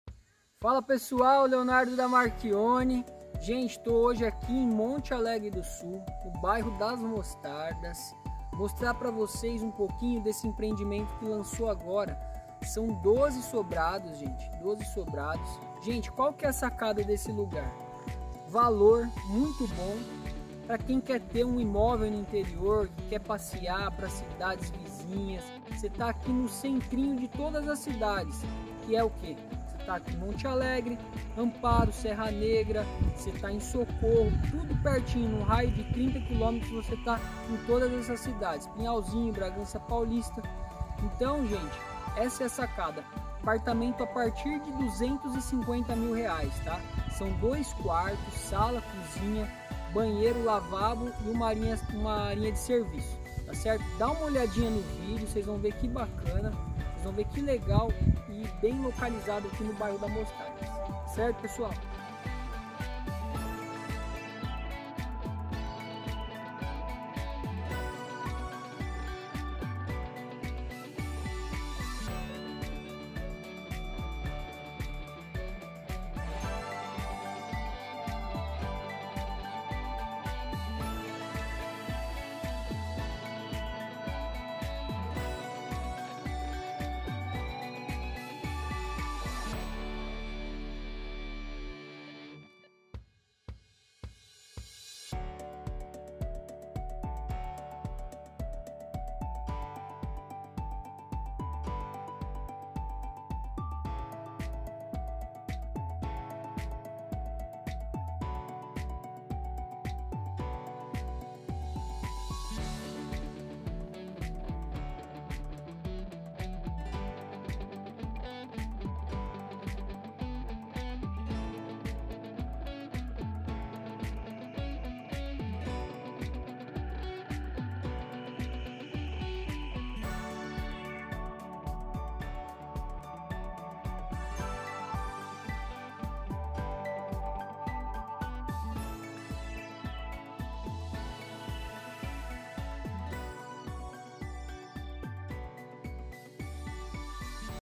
Condomínio de sobrados em Monte Alegre do Sul, bairro das Mostardas!🏡 a partir de 250 mil! ✔
Fala pessoal, tudo bem com vcs!?

Hoje estamos no bairro das Mostardas, cidade de Monte Alegre do Sul! Mostrar pra vcs esse condomínio de sobrados que está sendo lançado, são 12 apartamentos e vc que quer dar o pontapé no sonho de morar no interior, essa é uma opção acessível e bem localizada no circuito das águas!

Financiamento direto com o empreendedor ou bancário!

Outra sacada desse imóvel é fazer renda com a locação por temporada! Confira o vídeo e conheça um pouco mais deles!😉

Facebook.com/marchioninegociosimobiliarios
Instagram.com/marchioninegociosimob